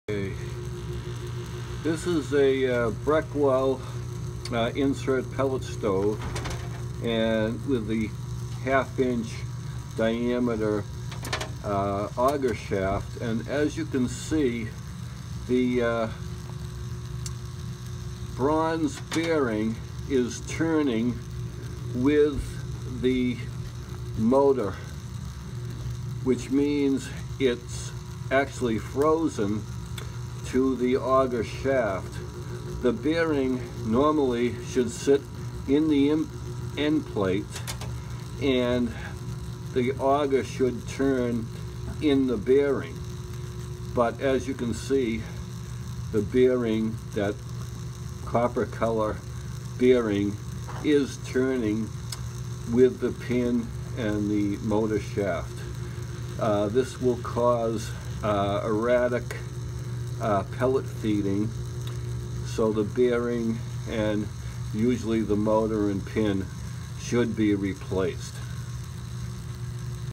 Breckwell 10 year old bronze bearing frozen to auger shaft - turns with auger motor!
Here is an example of why Pellets are jamming or feeding unevenly in a 10 year old Breckwell or other brand wood pellet stove oil lite bronze bearing frozen to the auger shaft that turns with auger motor and hitch pin. Bearing is gouged and lost all lubrication.
I have seen this happen on 1/2” auger shafts in Breckwell pellet stoves and 5/8” auger shafts in Avalon Pellet Stoves but never in stoves with a 3/4” auger shaft or stoves with high temp nylon or Nylatron bearings.